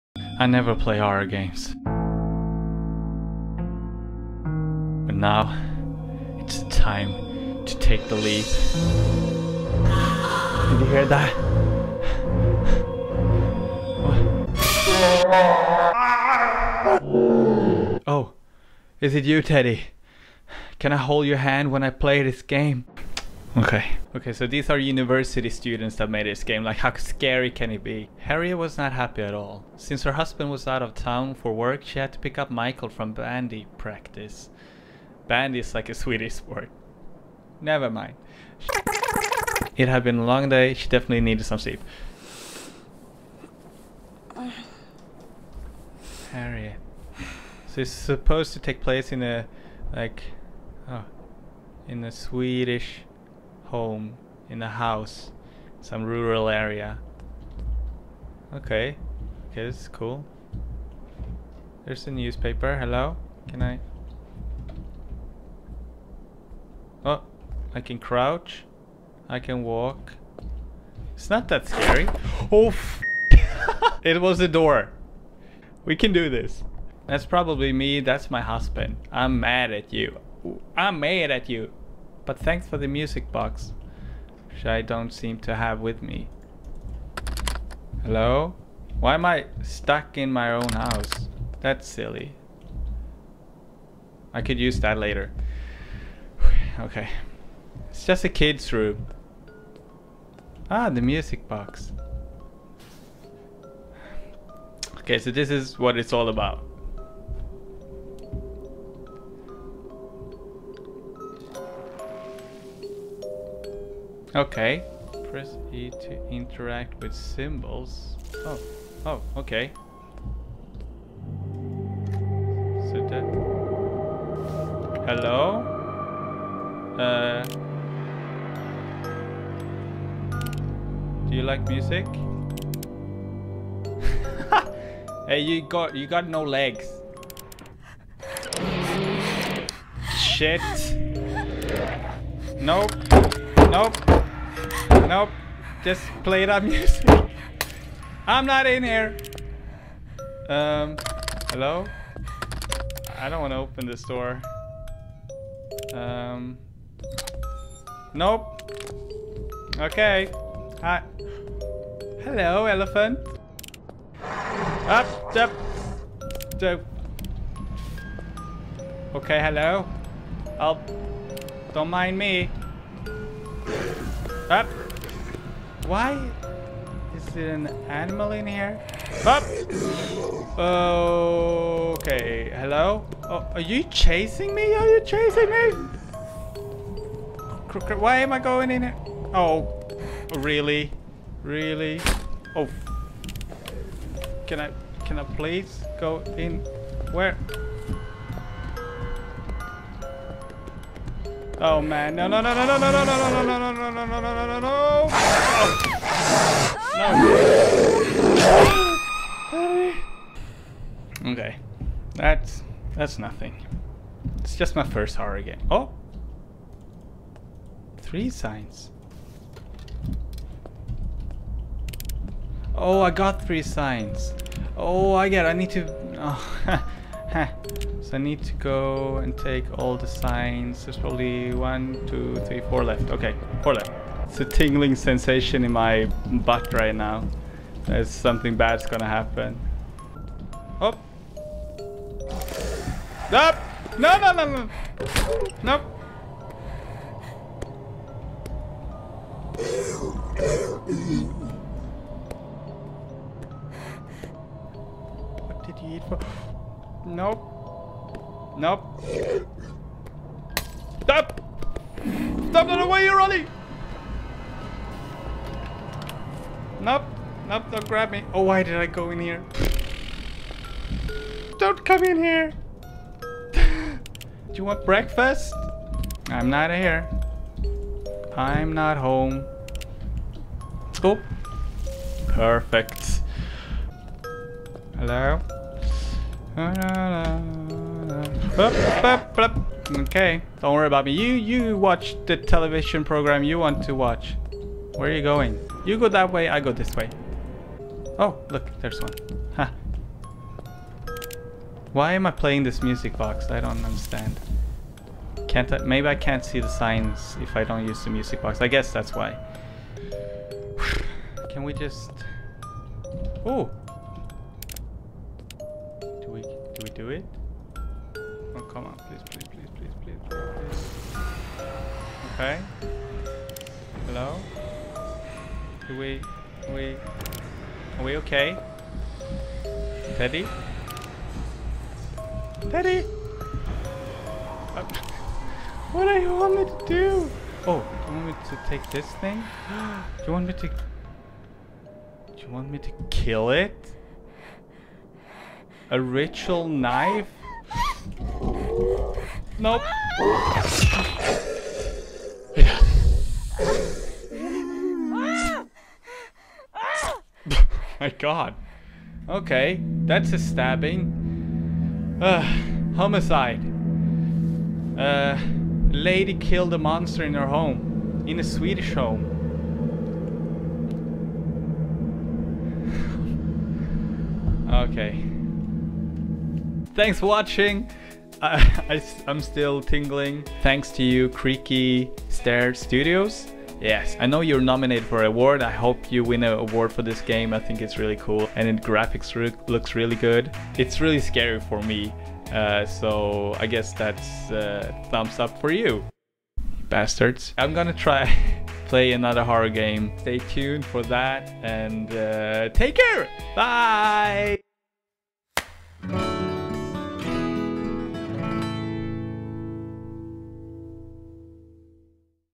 MY FIRST HORROR GAME
This is the first horror game I play! This game was developed by Creaky Stairs Studio, a small team of university students in Skövde, Sweden. This is the result of a project they worked on for 10 weeks. You can download the game for FREE